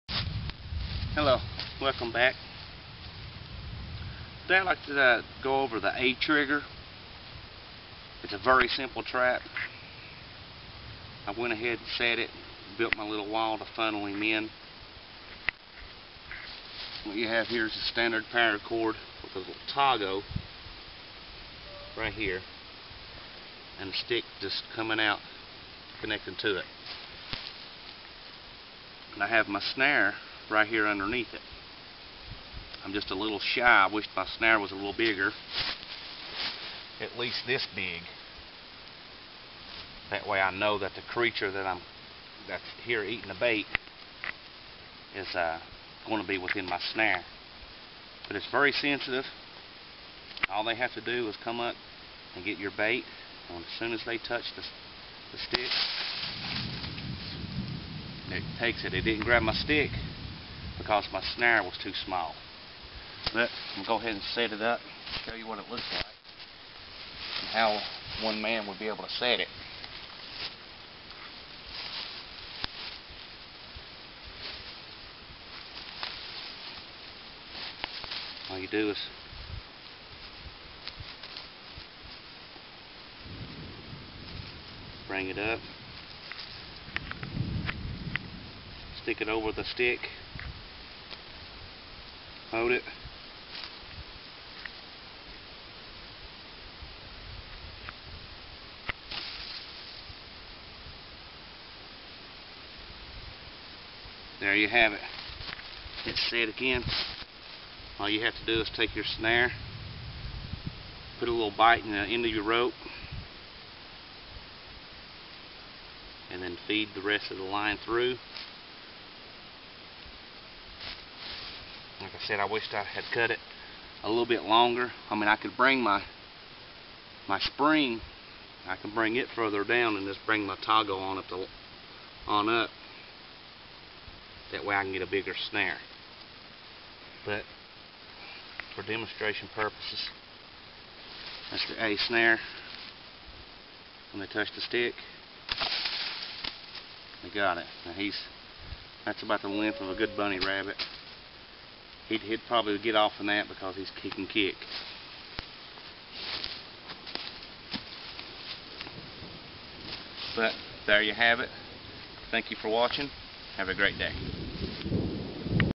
The A trigger figure snare
showing how to make the A Trigger Figure snare trap, This is one of my favorite prem traps to use because you can make several other different traps useing this same set up